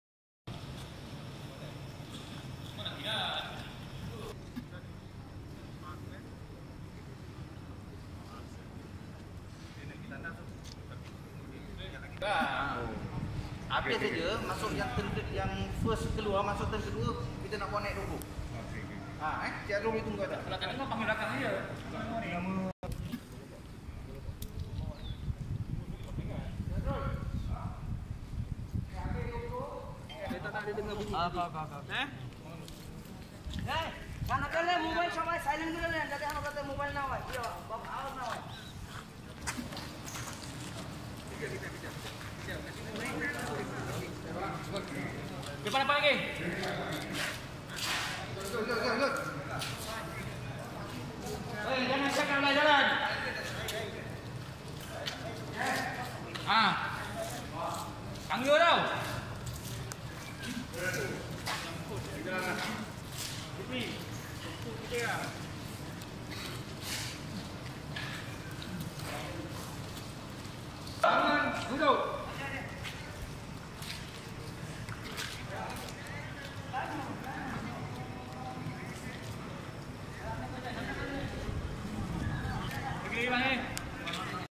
工厂天台集体祈祷·麻116外劳认罪各罰700
在行动管制令防疫期间，麻坡二南116名工厂外劳群聚在一家工厂的天台准备进行週五祈祷时，遭警方一网成擒。今日上午，这群外劳戴著口罩和手套，被带至麻坡法庭集体面控，全部承认有罪，每个人各罚款700令吉。
视频拍摄：蔡威亮